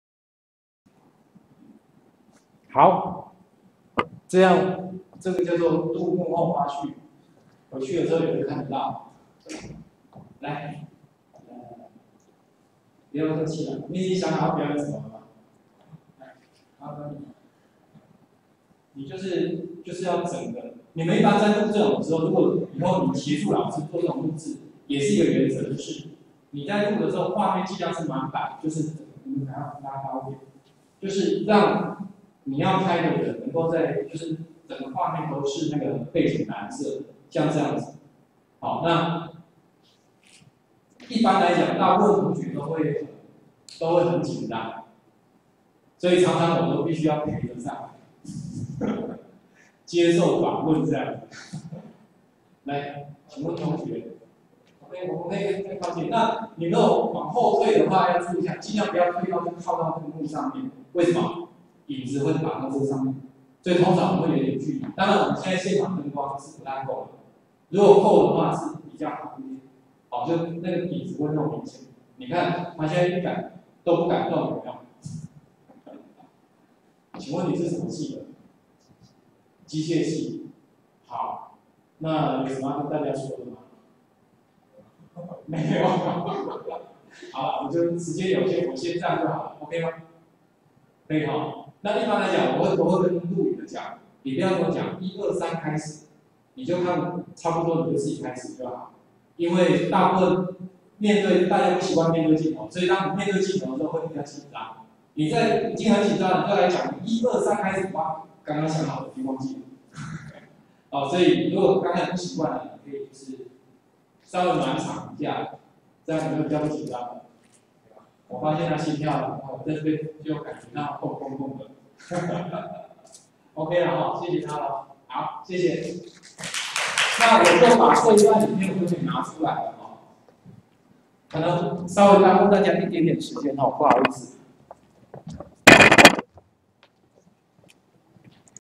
09：藍幕影片現場拍攝錄影與藍幕影片去背後比較
此研習錄影影片為「20161002中興大學TA：輕鬆搞定教學影片錄影剪輯(2016)
」的研習錄影！更多詳細訊息請參閱：述文老師部落格

非常感謝同學今天的參與，我們研習的錄影影片己經處理好，歡迎同學有空上網複習！

另外，今天有拍攝的藍幕影片，老師已經將它利用威力導演處理好，讓同學在這段影片中可以清楚看到拍攝現場、原始藍幕影片與去背後的藍幕影片效果！(不過，因為同學錄影時畫面有裁切到部份人頭影像，效果會比較差一些，建議日後同學要錄影時，請記得除了滿版的藍色外，最好將拍攝的主題放在畫面正中間喔！

最後，因為今天時間很有限，建議同學可以參考老師 05/07、05/14 這兩天在東海大學TA研習「原來數位教材編製與錄影 一個人就能輕鬆完成(2016)」，第一天是以 Debut Video Capture 與 ActivePresenter 為主，第二天則是以威力導演為主，可以讓同學更完整了解到這些軟體的操作與應用技巧！